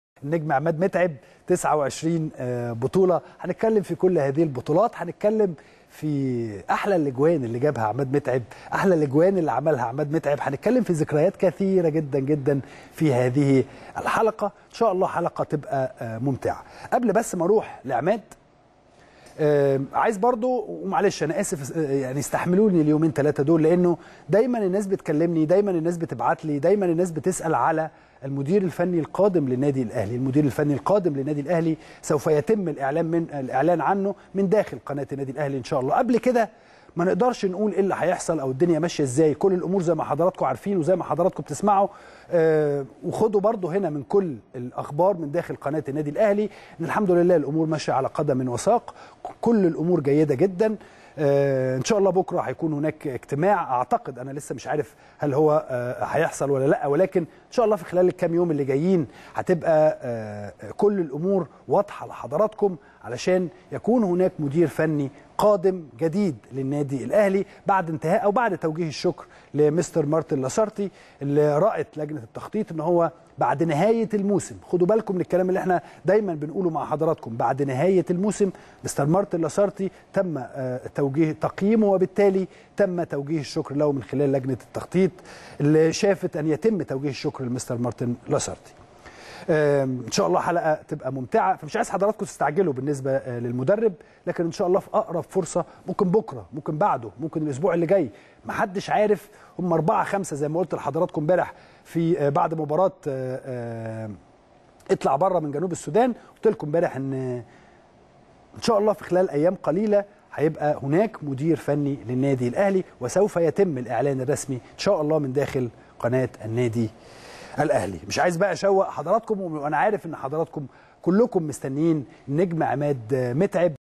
اسلام الشاطر : المدرب الجديد لن يتم اعلانه الا من خلال شاشة الاهلى
التردد: 12341 أفقي.
القمر: نايل سات.
معامل الخطأ: 5/6.
معدل الترميز: 27500